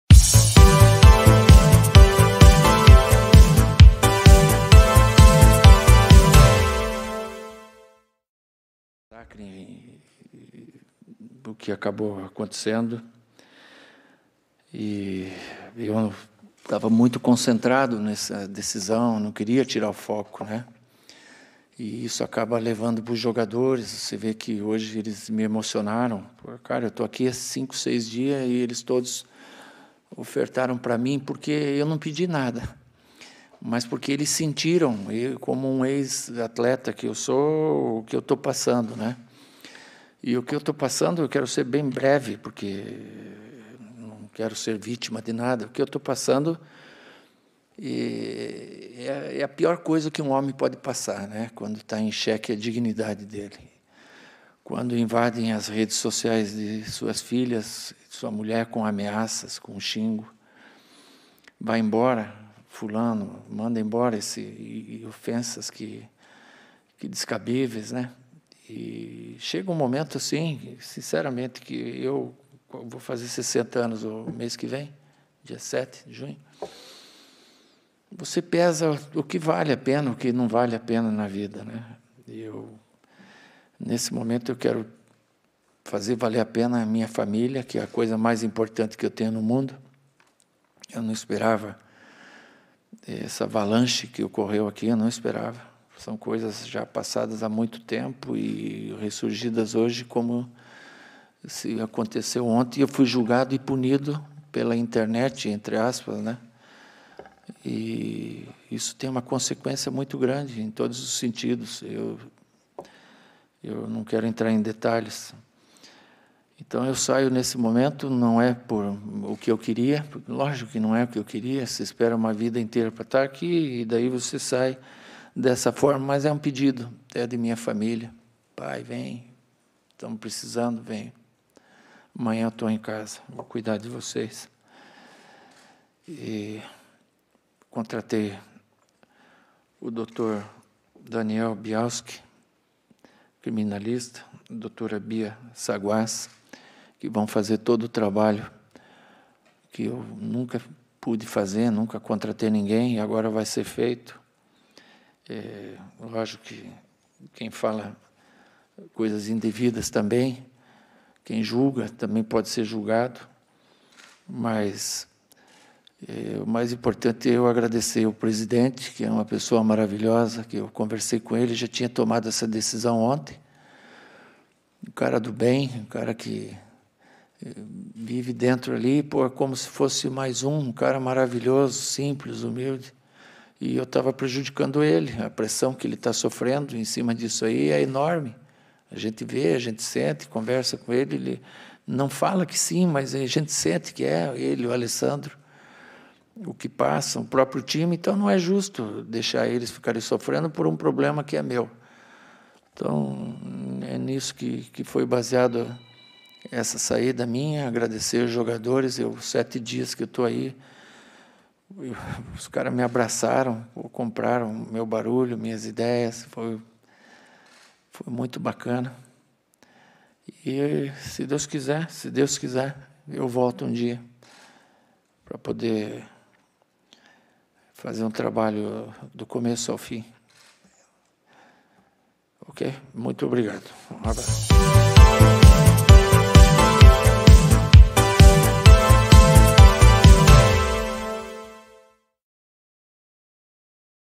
Cuca deixa o comando do Corinthians após dois jogos
Decisão foi anunciada após a partida contra o Remo, nessa quarta-feira; técnico cita família e pressão nas redes como principais motivos: "Amanhã estou em casa"